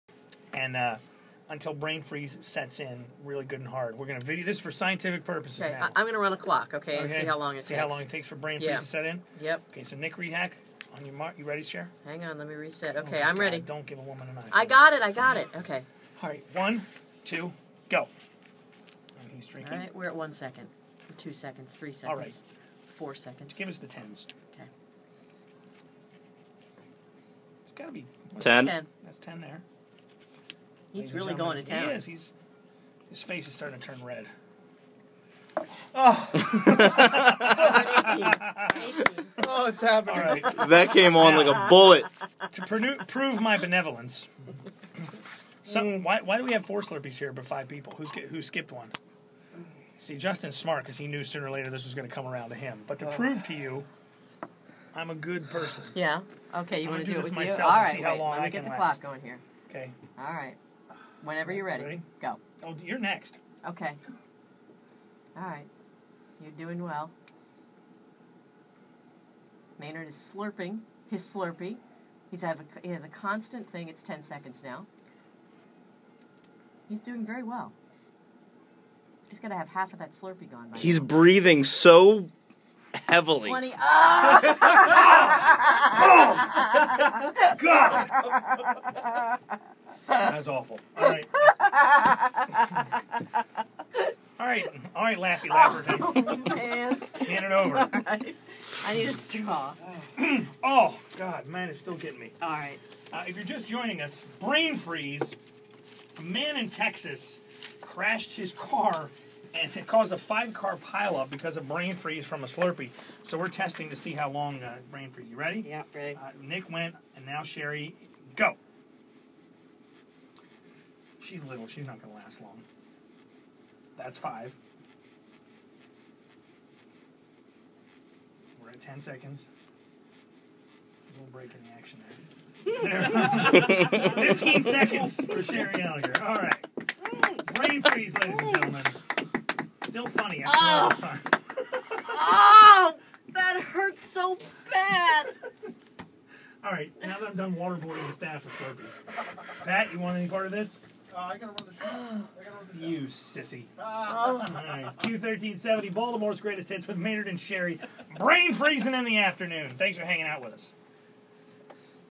Brain Freeze!!!!
A guy in Texas caused a 5 car pile up due to Brain Freeze from his Slurpee.

We decided to Brain Freeze ourselves!